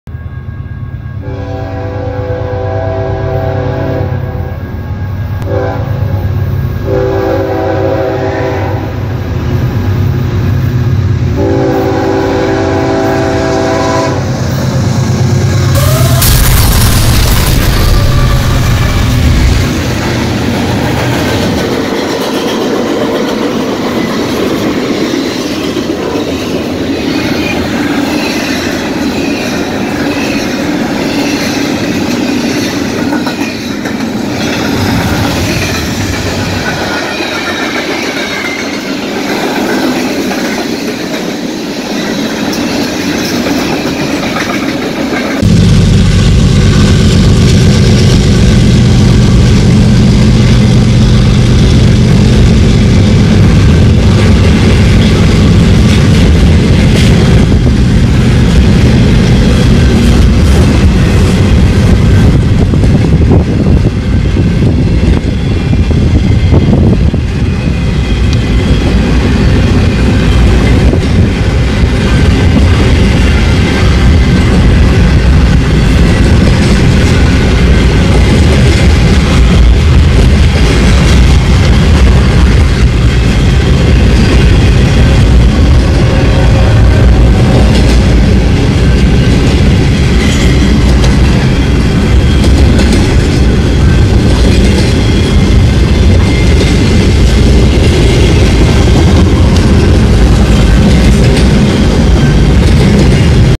Another Train Wreck In Johnston, South Carolina
Today, NS train 205 was traveling from Linwood, NC to Macon, GA. The train departed Linwood Yard at 6:44am =. Just after 9:40am, the train was approaching Johnston, South Carolina. But there was trouble, a semi truck got stuck on the tracks at Lee Street